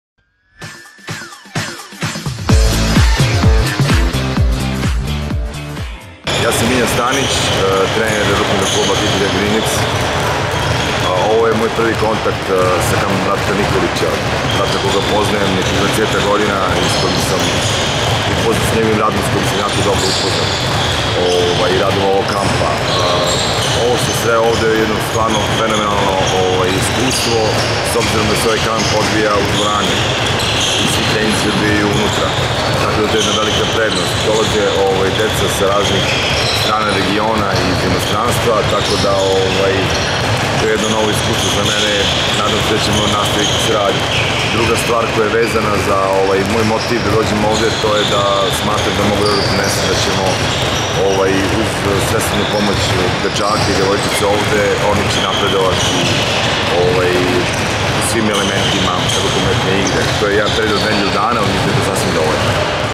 Ukratko sa Miljanom Stanićem
Miljan Stanić je novi trener našeg kampa, i predstavlja pravo osveženje. Nekada je bio igrač, a sada je trener sa zanimljivim međunarodnim iskustvom. Nekada je igrao u Tunisu, Portugalu, Izraelu i Islandu, a uspešnu međunarodnu karijru je izgradio u Izraelu, sada je superligaški trener u Srbiji